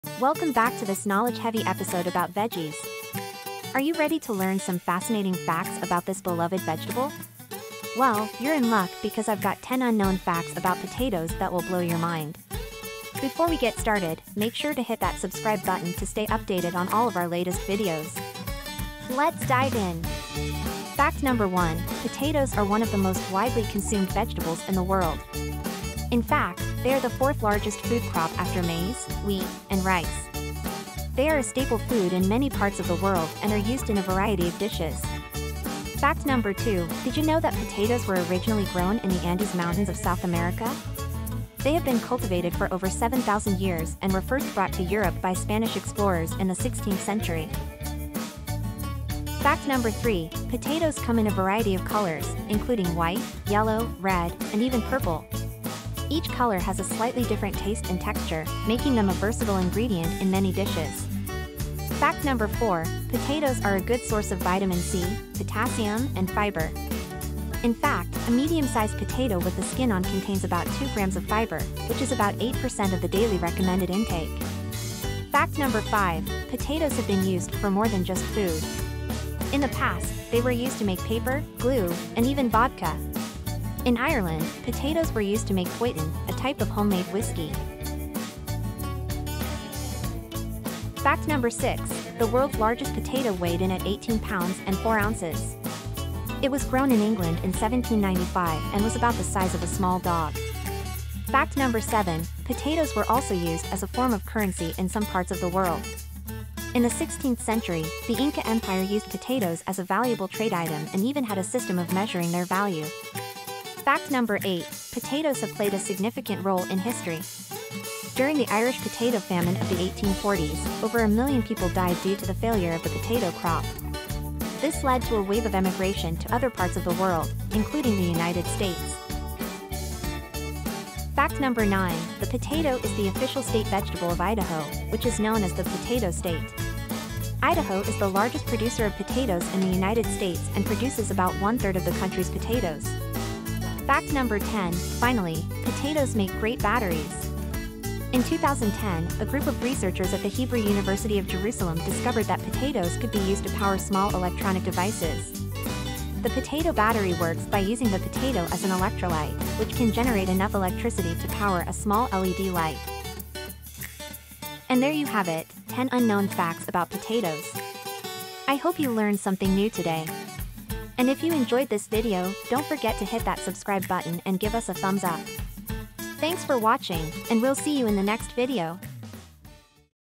10 Surprising Facts About Potatoes You Need to Know
In this video, we're going to be sharing ten fascinating and unknown facts about potatoes that will leave you amazed. From their origins in the Andes Mountains to their use as a form of currency, potatoes have a rich history that you may not know about.

We'll also be sharing some nutritional information about potatoes and even how they can be used as batteries! So, if you're a fan of this versatile vegetable, be sure to tune in and learn something new.